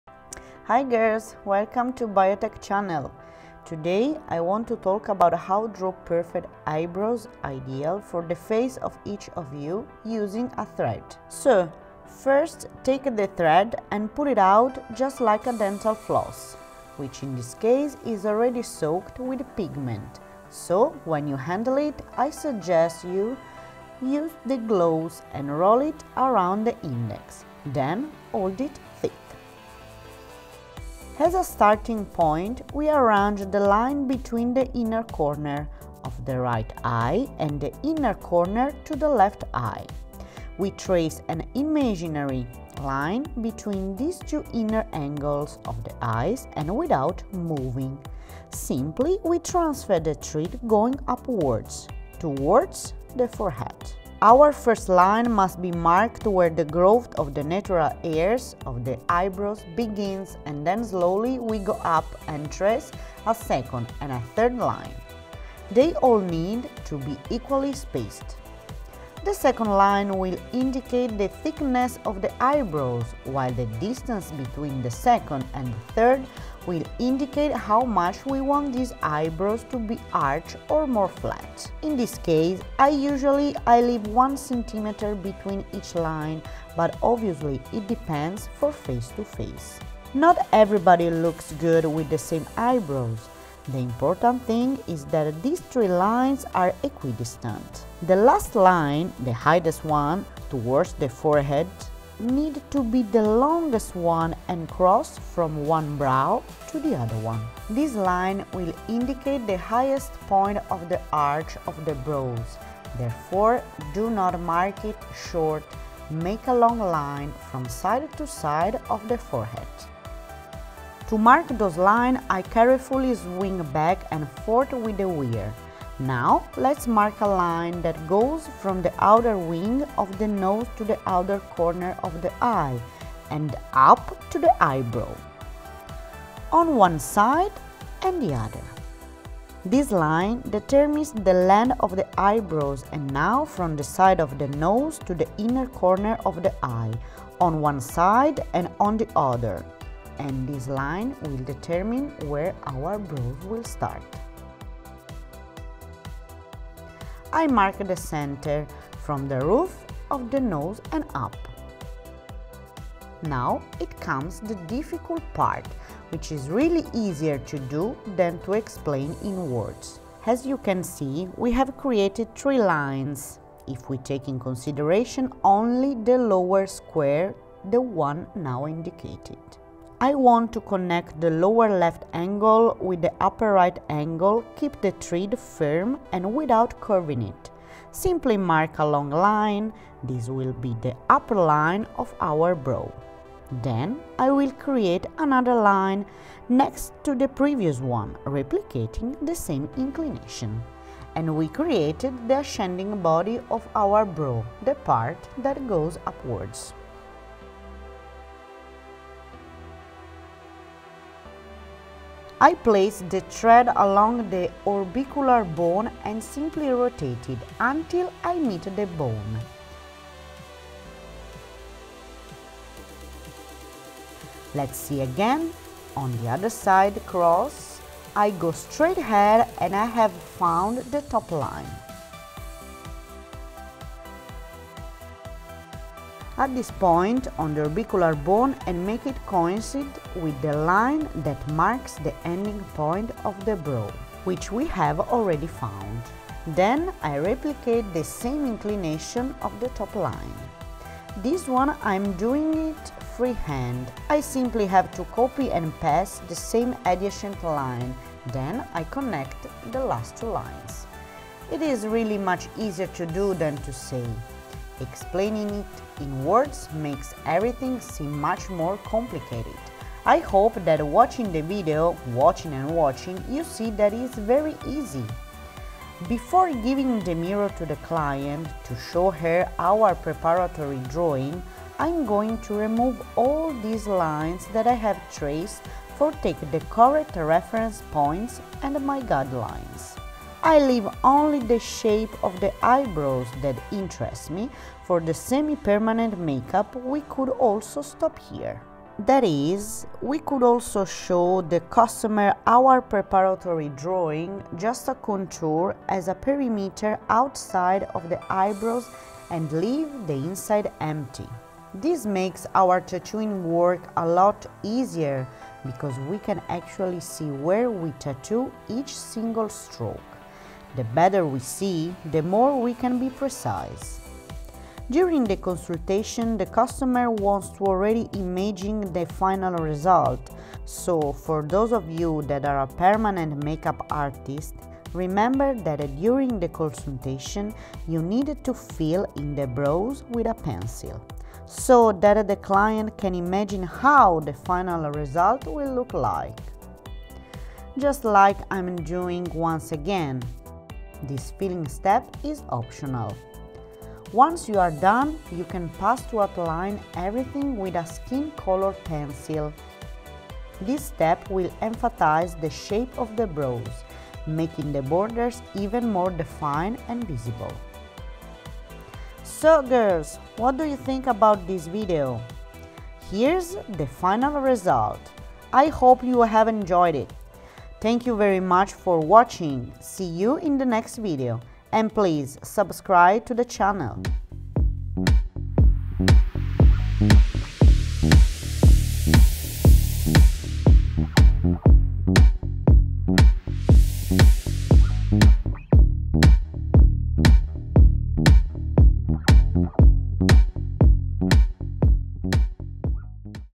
HOW TO DRAW PERFECT EYEBROWS | PreDrawing Permanent Makeup
Designing eyebrows suitable for every woman's face is not easy and we, as experts in dermopigmentation, know it well because before each semi-permanent make-up session we have to design the shape of the eyebrows that we are going to tattoo on the client.
There are different ways to carry out this difficult phase, with the help of technical tools that make it easier for us to mark the points of reference for our preparatory drawing on the skin.
There are many ways to carry out the preparatory drawing:
1. with the preparatory drawing pen

2. with the caliper

3. with the thread

in this video I will show you how to make the preparatory drawing with a pigment-soaked thread.
Good vision!

PS: it is much easier to do than to say!!